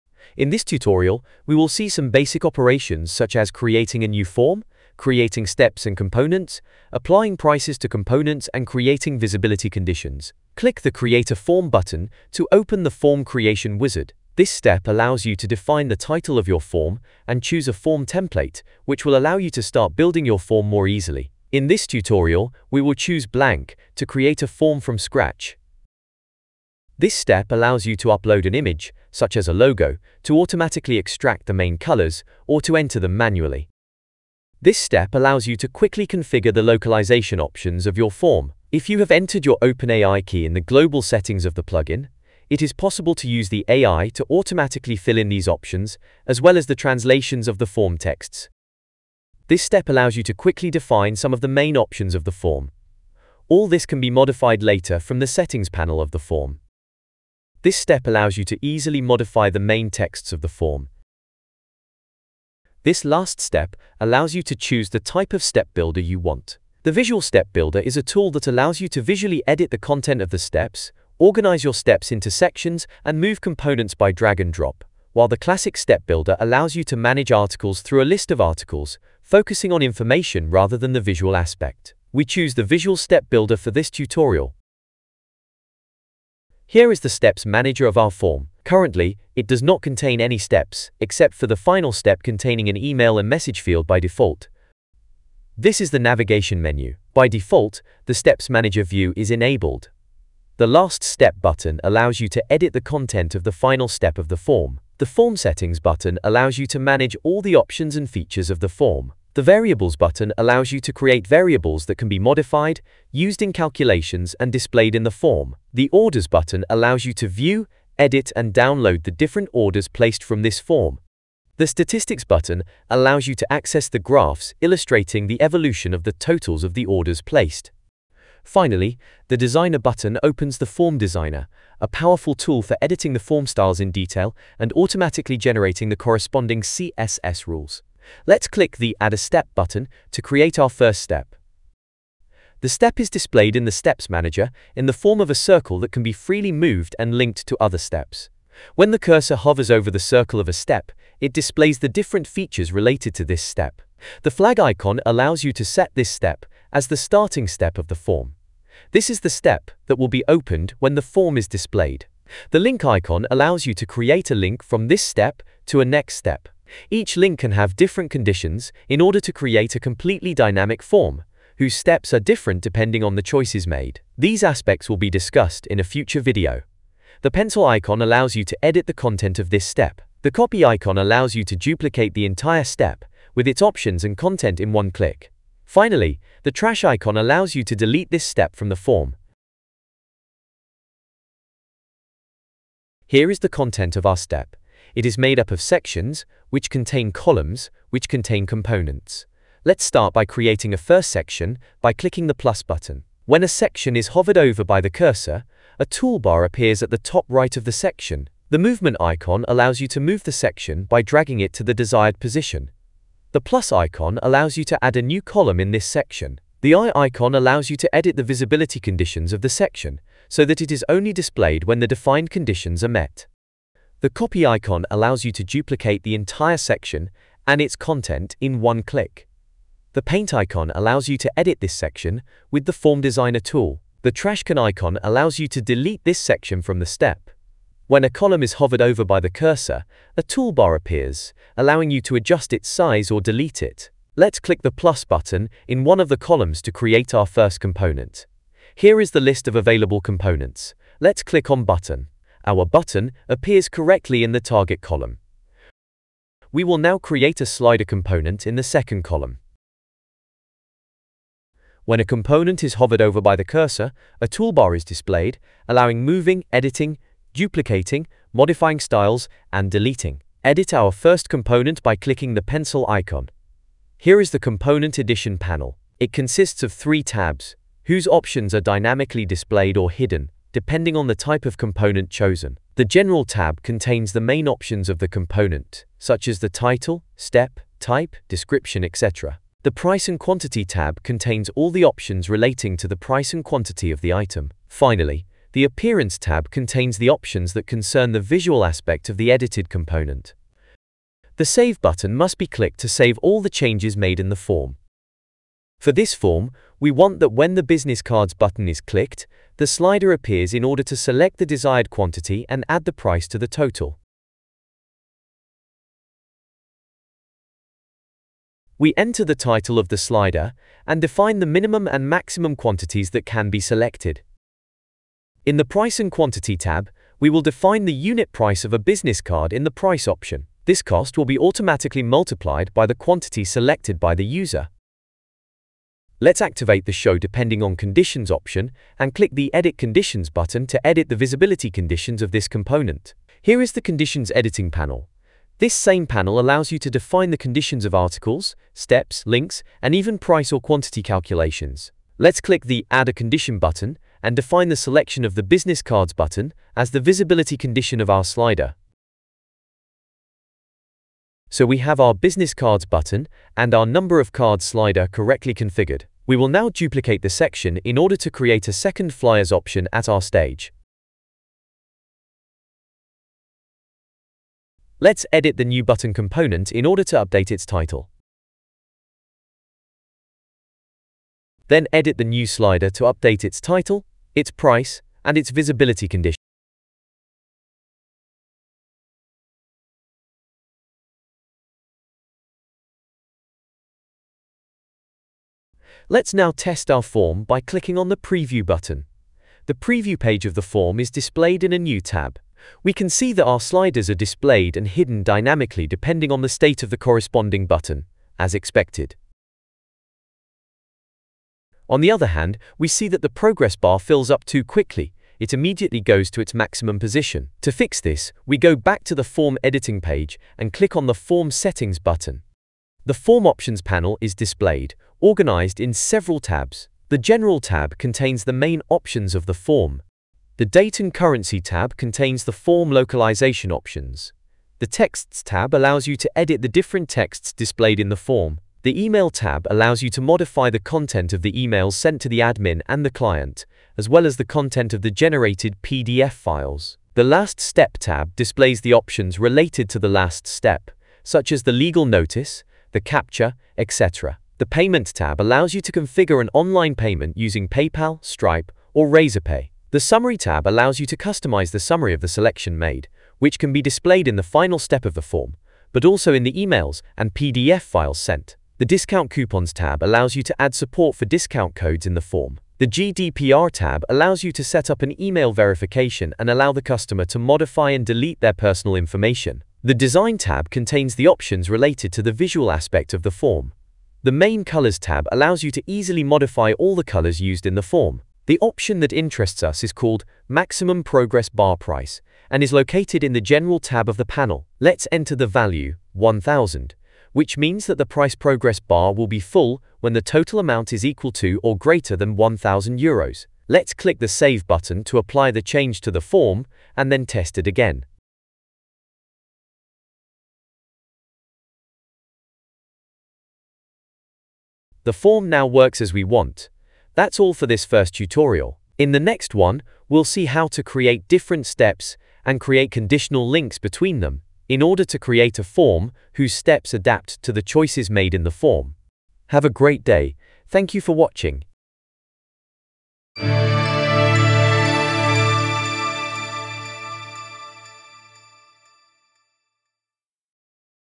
Create your first form - Tutorial for Cost Estimation & Payments Forms Builder
This tutorial shows how to create a form, a step, manage its components, use prices and visibility conditions.

This is a tutorial for the legendary WP Cost Estimation & Payments Forms Builder plugin, sold over 21K and rated 5/5 by thousands of customers